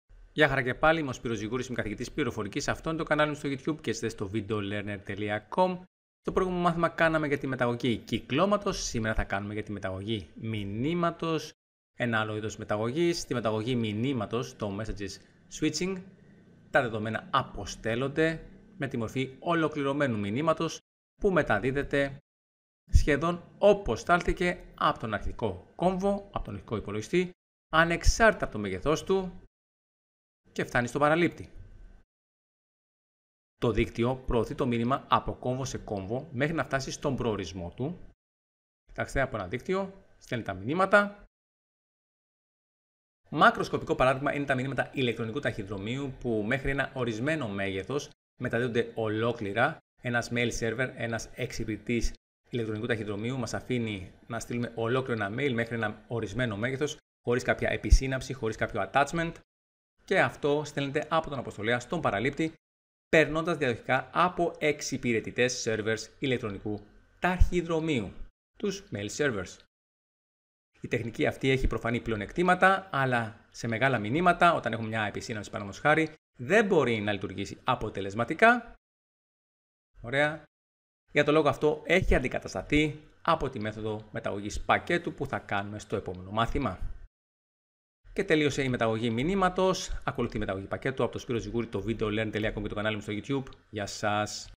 Μεταγωγή Μηνύματος
Υλικό & Δίκτυα Υπολογιστών
Στη μεταγωγή μηνύματος (messages switching) τα δεδομένα αποστέλλονται
με τη μορφή ολοκληρωμένου μηνύματος που μεταδίδεται
σχεδόν όπως στάλθηκε από τον αρχικό κόμβο,
ανεξάρτητα από το μέγεθός του.
Το δίκτυο προωθεί το μήνυμα από κόμβο σε κόμβο
μέχρι να φτάσει στον προορισμό του.
Μακροσκοπικό παράδειγμα είναι τα μηνύματα ηλεκτρονικού ταχυδρομείου που
μέχρι ένα ορισμένο μέγεθος μεταδίδονται ολόκληρα
από τον αποστολέα στον παραλήπτη
περνώντας διαδοχικά από εξυπηρετητές ηλεκτρονικού ταχυδρομείου.
Η τεχνική αυτή έχει προφανή πλεονεκτήματα
αλλά σε μεγάλα μηνύματα
δεν μπορεί να λειτουργήσει αποτελεσματικά.
Για το λόγο αυτό, έχει αντικατασταθεί από
τη μέθοδο μεταγωγής πακέτου.